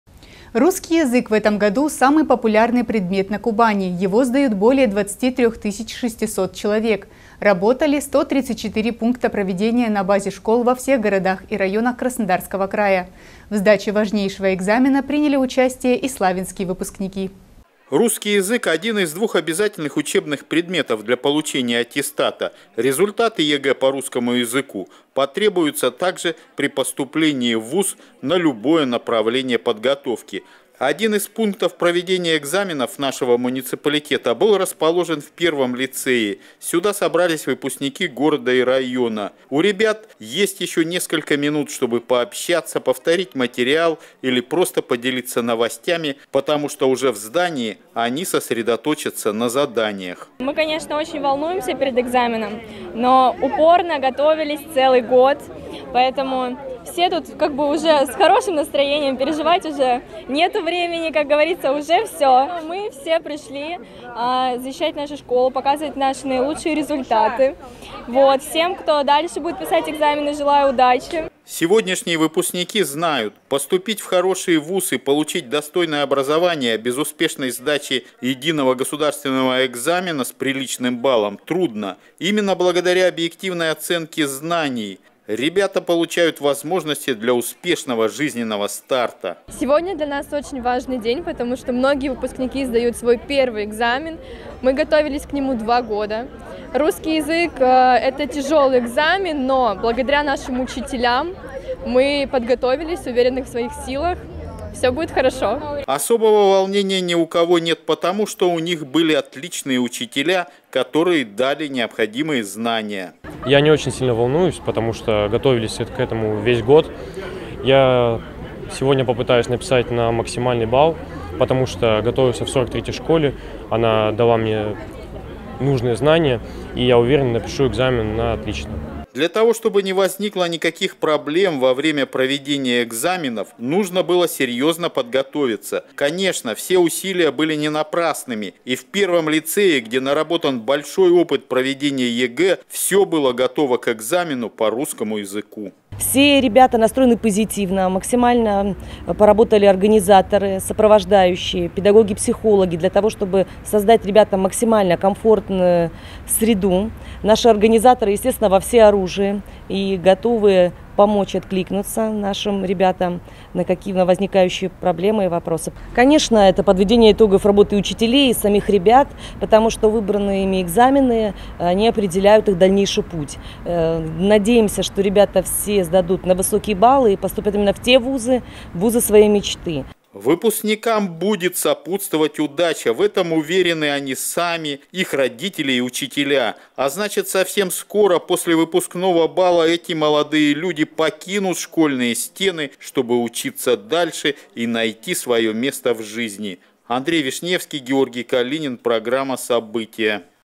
ЕГЭ по русскому языку
Русский язык в этом году — самый популярный предмет на Кубани. Его сдают более 23 тысяч шестисот человек. Работали 134 пункта проведения на базе школ во всех городах и районах Краснодарского края. В сдаче важнейшего экзамена приняли участие и славянские выпускники